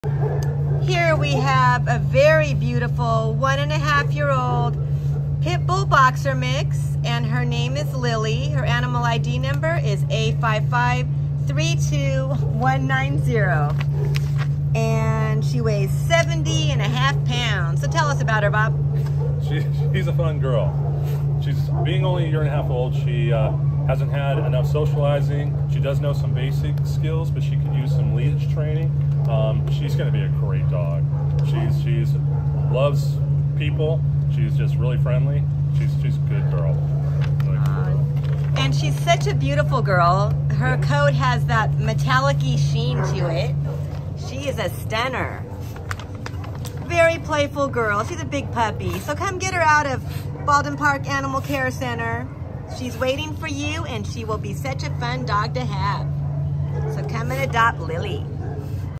New! A5532190 Lily | Pit Bull/Boxer
A5532190 Lily is a nice 1 ½ year old coffee and white spayed female Pit Bull and Boxer mix who came to the Baldwin Park Animal Care Center on February 5 after being found as a stray. Weighing 70 lbs, Lily is curious to get out of her kennel and explore the world. She’s a strong girl who hasn’t had much training so she needs someone who can handle her spontaneous pulls on leash. The shelter says she does okay with other large dogs and has a push/pull play style. Sometimes she gets vocal when she plays and is having fun. Lily needs socialization and a group class would be great for her both for training and to build her confidence around other dogs. With huge feet we think she might get a little heavier, as she s still growing into her frame, and her face has the look of a Boxer. Lily is treat motivated and already knows sit. With floppy ears and a waggy tail, Lily is a happy young dog ready to live a fun life with anyone who is committed to teaching her what she needs to know, and who has patience for a large teen dog.